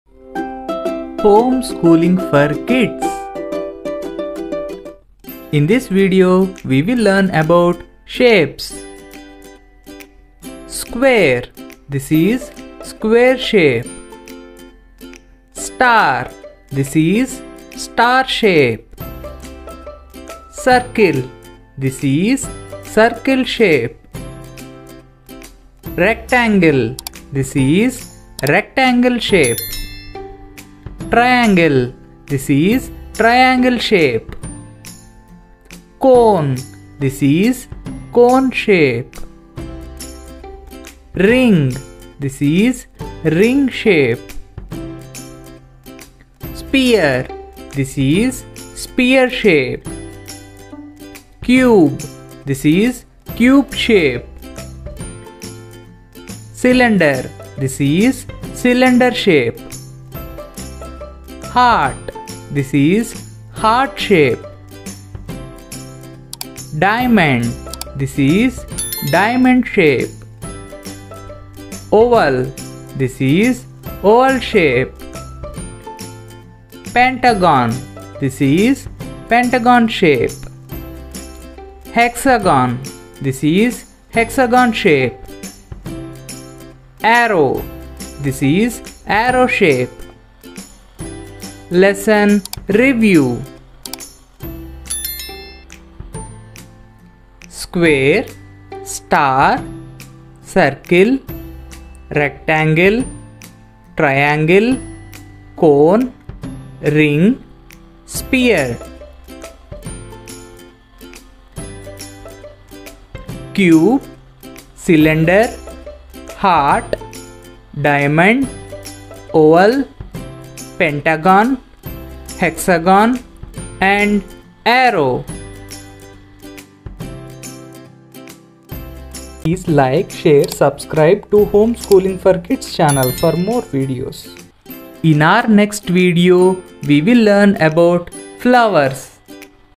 Learn Shapes with examples | Spellings | Home Schooling for kids | Vd:9
Home Schooling for kids

In this video we will learn Shapes with examples, spellings, shapes, effective english vocabulary in English

Shapes
Content creator : Dinesh